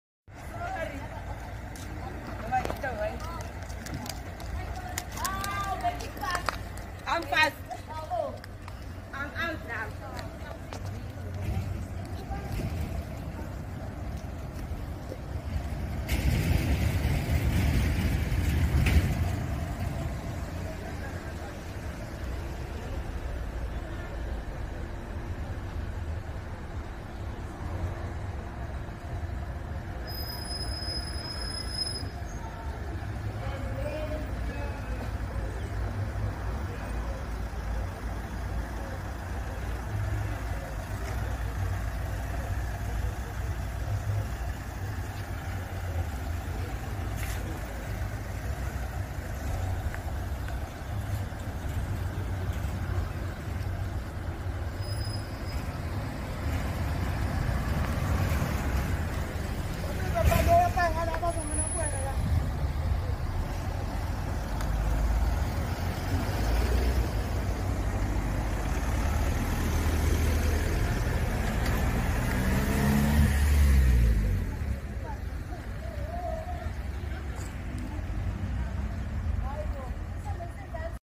Mbongeni Ngema's remains arriving in Durban from Eastern Cape
If there is a copyright issue or if we have posted a video of yours and you would like us to take it down, please feel free to reach out to us through email.